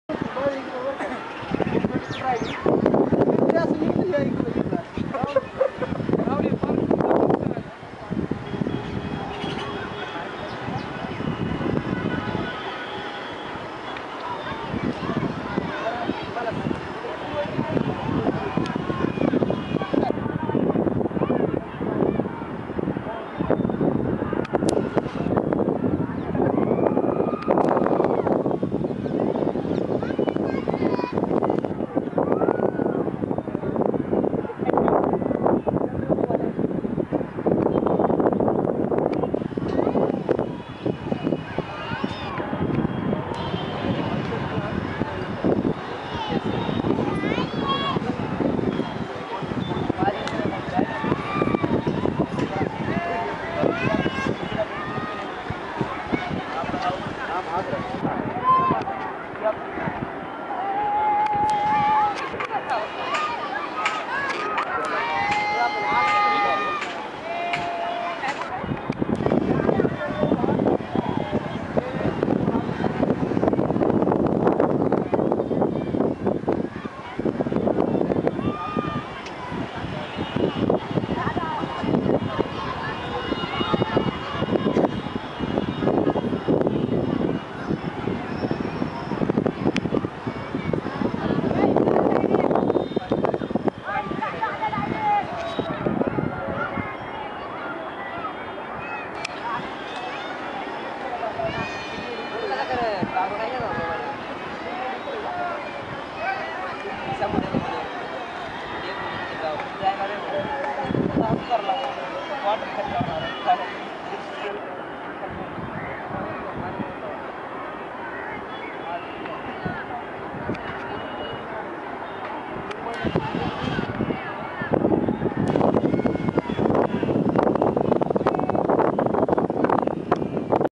pune from back side of parvati temple
u can see pune surrondings from here
taken by : Sandeep and Sudeep Kumar